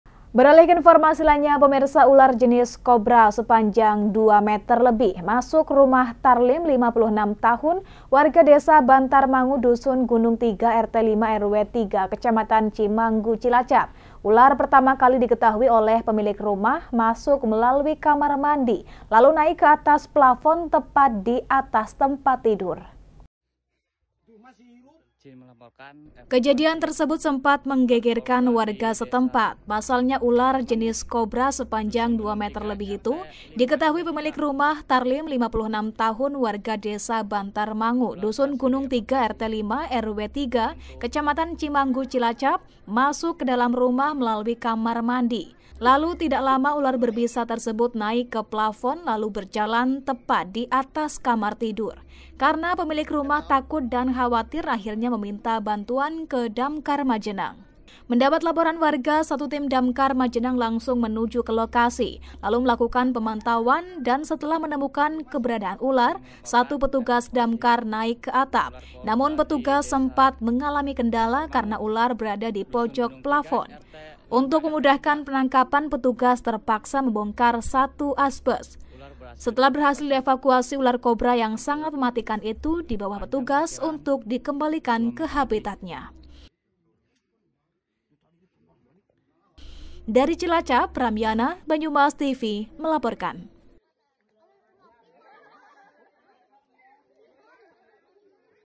ULAR COBRA SEPANJANG 2 METER LEBIH MASUK PLAFON RUMAH WARGA
ULAR JENIS COBRA SEPANJANG 2 METER LEBIH MASUK RUMAH TARLIM  (56 TAHUN) WARGA DESA BANTARMANGU DUSUN GUNUNG TIGA RT 05 RW 03 KECAMATAN CIMANGGU CILACAP. ULAR PERTAMA KALI DIKETAHUI OLEH PEMILIK RUMAH MASUK MELALUI KAMAR MANDI LALU NAIK KEATAS PLAFON TEPAT DIATAS TEMPAT TIDUR. SETELAH BERHASIL DIEVAKUASI TIM RESCUE DAMKAR MAJENANG, ULAR BERBISA TERSEBUT LALU DIAMANKAN UNTUK DIKEMBALIKAN KEHABITATNYA.